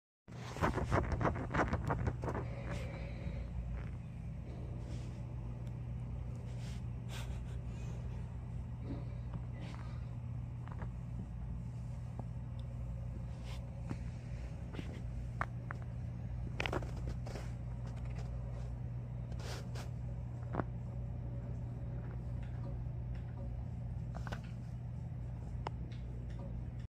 oh okay, bye I guess...
I thought Chai wanted to come help me edit. Instead she just wanted to go past me and crawl into the chair. Okay I guess. Ferrets are weird.

Love the animal shenanigans?  I also made a nice wish list for the pets!